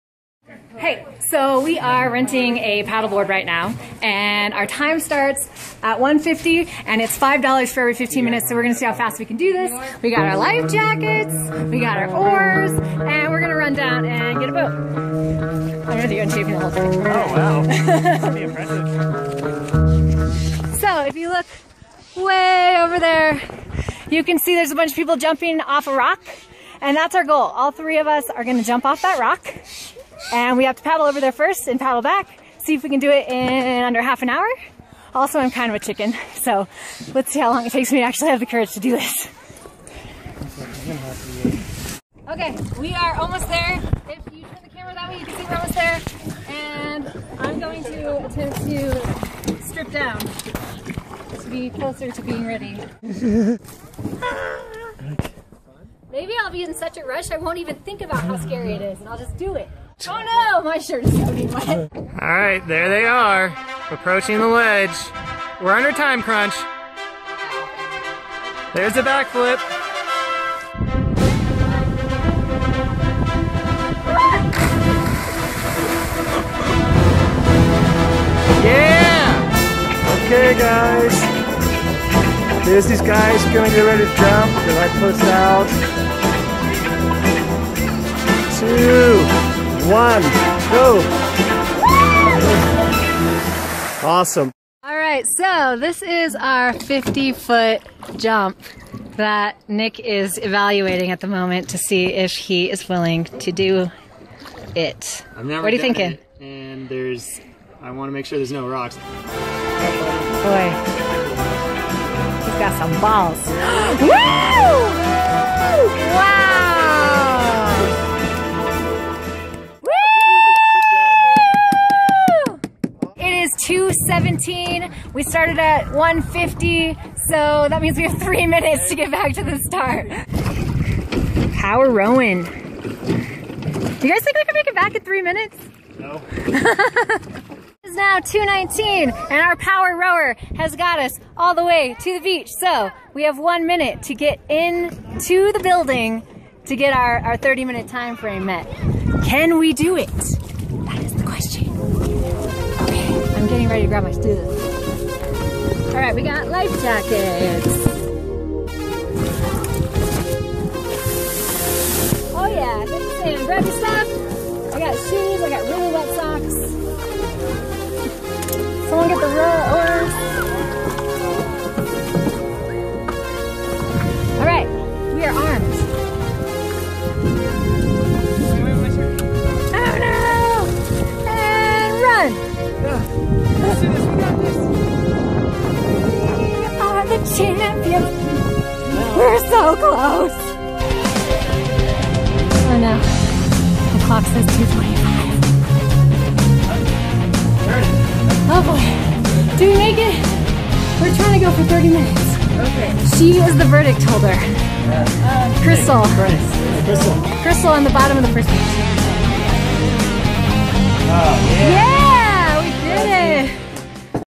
cliff jumping
We did a timed boat rental and cliff jumping at Angora Lakes for my Live Life Loud VLOG.